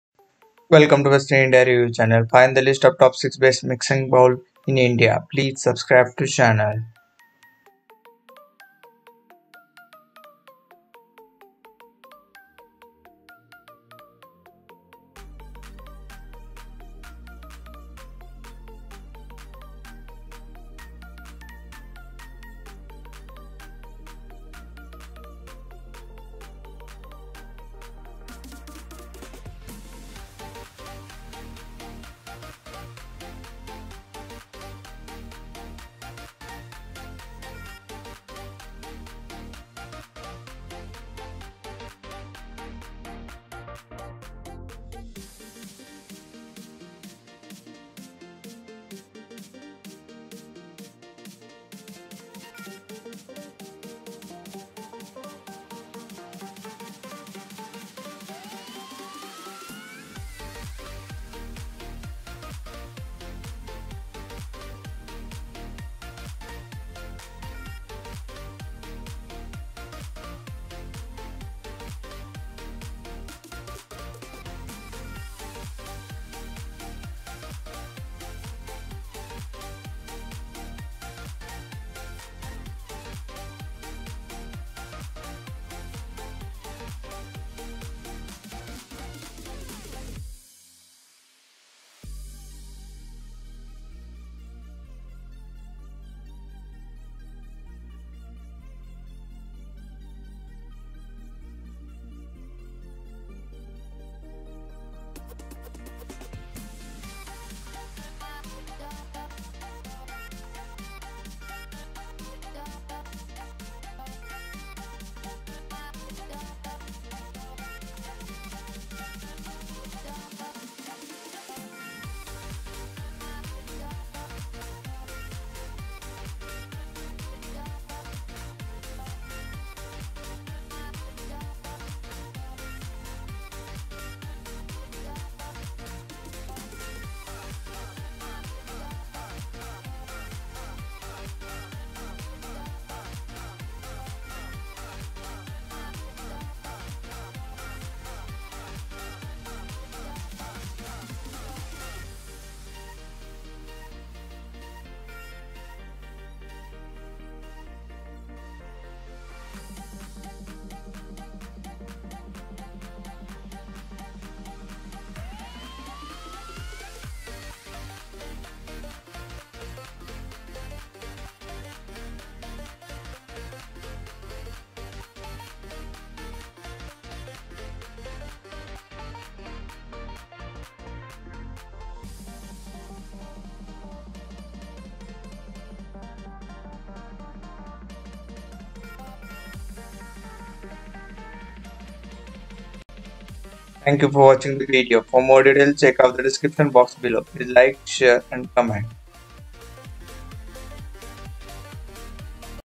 Top 6 Best Mixing Bowl in India 2021 | KITCHEN BOWL SET | MIXING BOWL FOR BAKING | मिश्रण का कटोरा
Top 6 Best Mixing Bowl in India 2021

List of Top 6 Best Mixing Bowl in India

1. Kitchen Kemistry Solid Mixing Bowl Set

2. SignoraWare Mixing Bowl

3. UNIFOX Stainless Steel Mixing Bowls

4. Cello Ornella Mixing Bowl Set

5. All Time Plastics Classic Mixing Bowl Set

6. Jaypee Plus, Mixing Bowl
Other Kitchen:

You want to buy a Best Mixing Bowl Online then here is the list of Top 6 mixing bowl for baking.
What is the best material for mixing bowls?
What are the best glass mixing bowls?